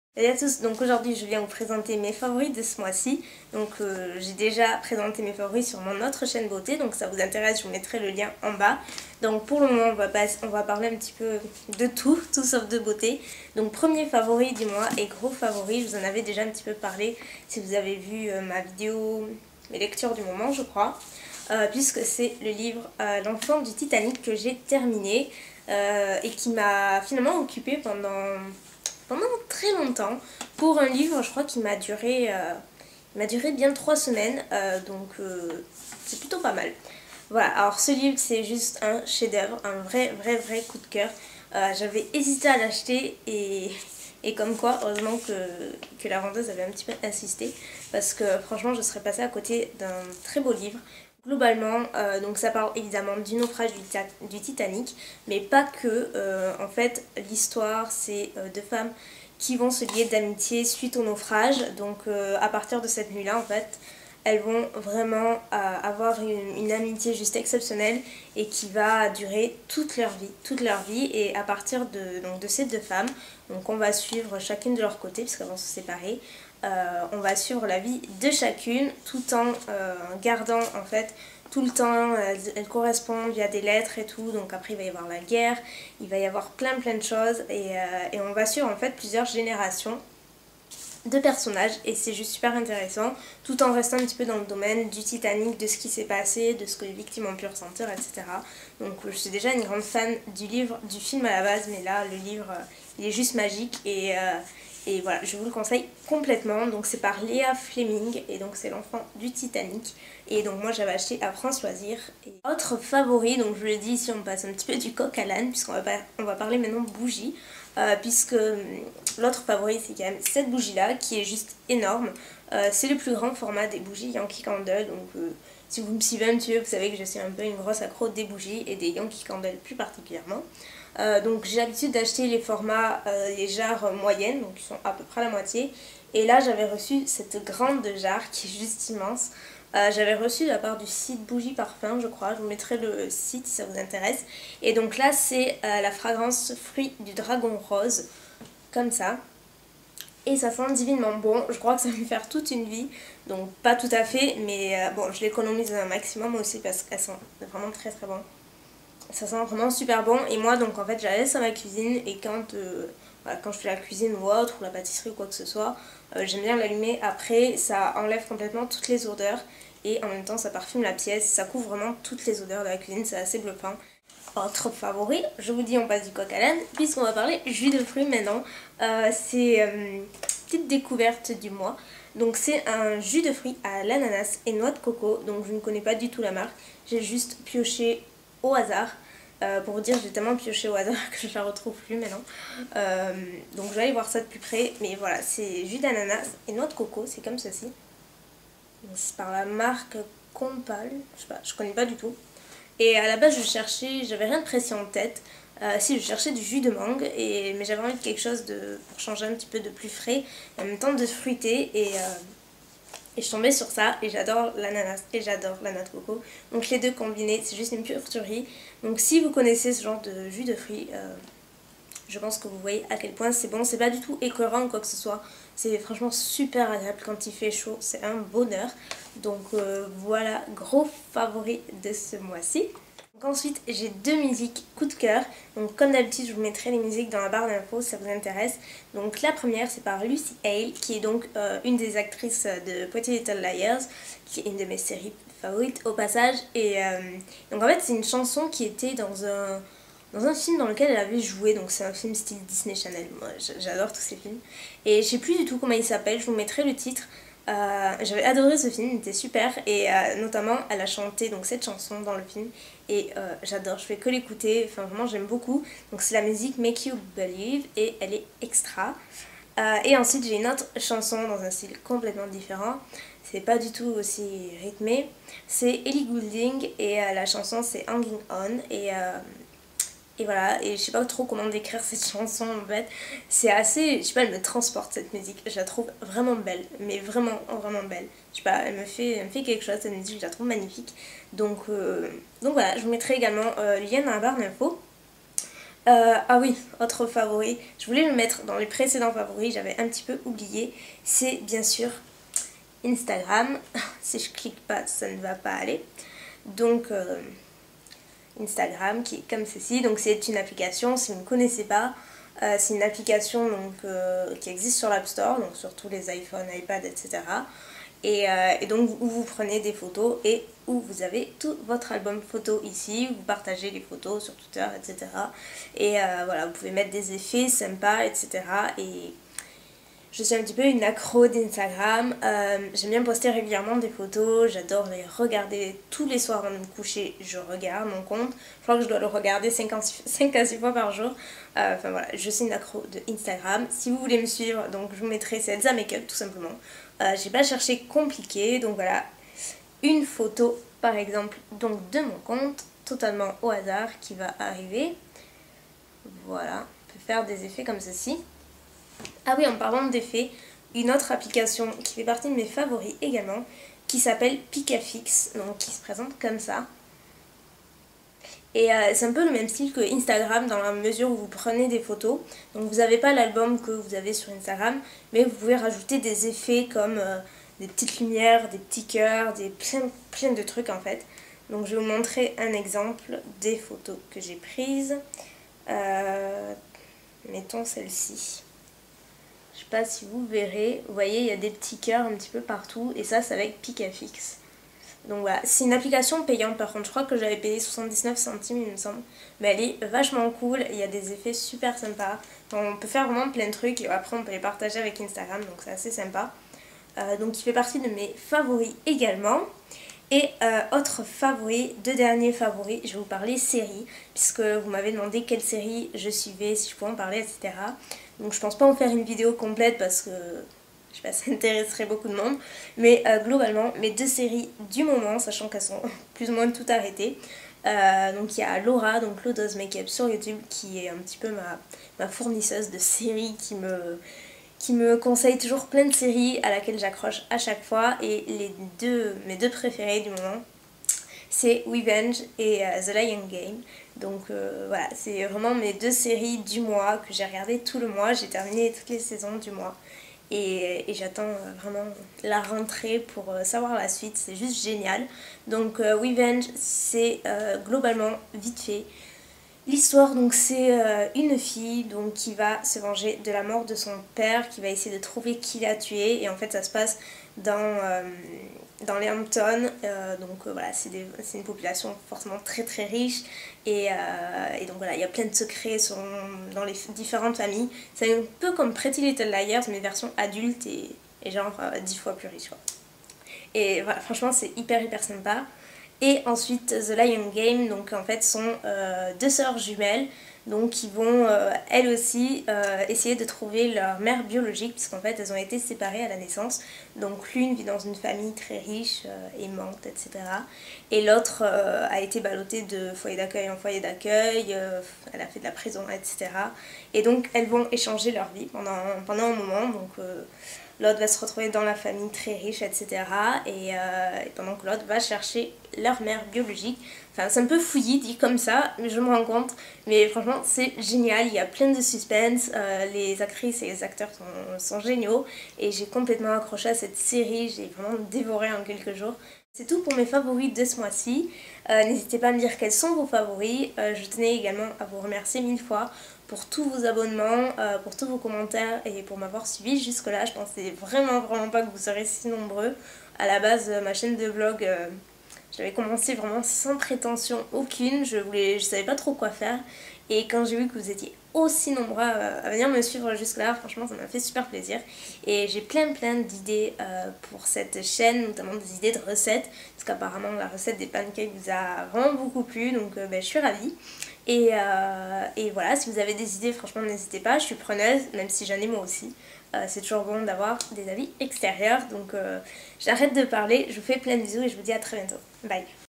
Mes Favoris (Juillet)
♡ Séries mentionnées :
- Revenge
- The Lying Game

♡ Ce que je porte :
Top : Forever 21
Bague : Forever 21
Rouge à lèvre : Cut a Capper - Mac + Gloss Nude Peach - NYX
Vernis : Flip Flop Fantasy - China Glaze